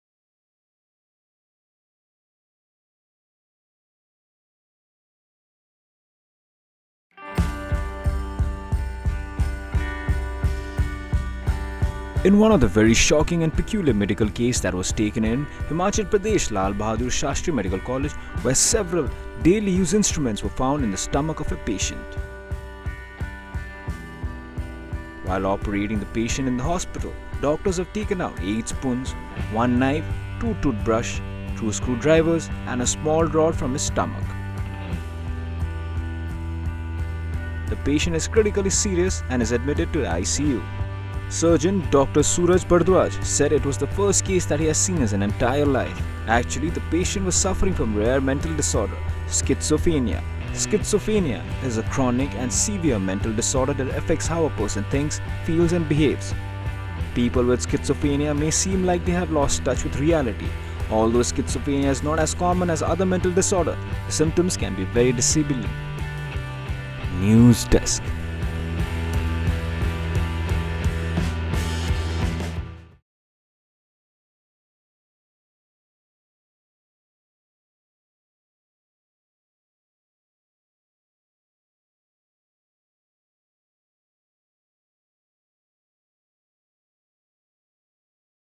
Spoons, screwdriver, knife & rod taken out from the stomach after 4 hrs of operation
In one of the very shocking and peculiar medical case that was taken in Himachal Pradesh's Lal Bahadur Shastri Medical College where several daily use instruments were found in the stomach of patient. While operating the patient in Lal Bahadur Shashtri medical college doctors have taken out 8 spoons, one knife, two toothbrush, two screwdrivers and a small rod from his stomach. The patient is critically serious and is admitted in the ICU.